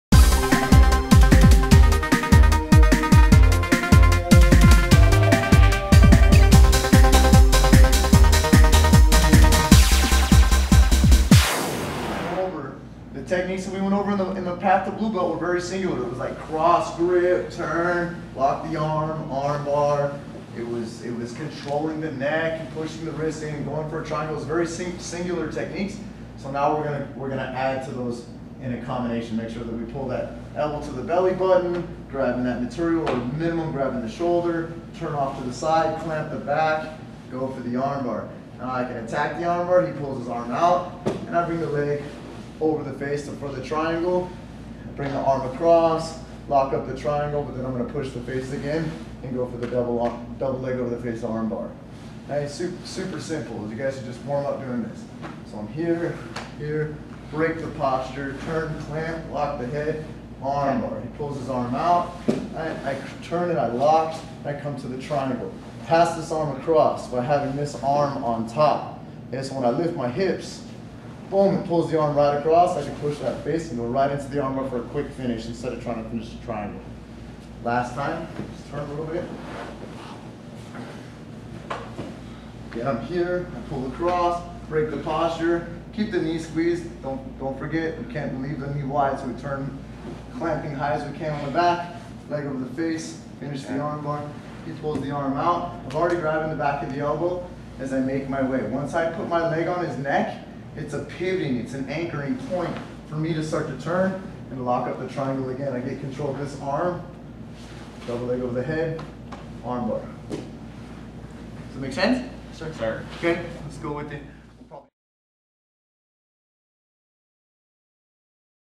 BJJ Armbar Triangle Armbar Drill
Brazilian Jiu Jitsu Black Belt, Brandon Quick, owner and chief instructor for the BQuick Jiu Jitsu Network and Team Trinity demonstrating grappling techniques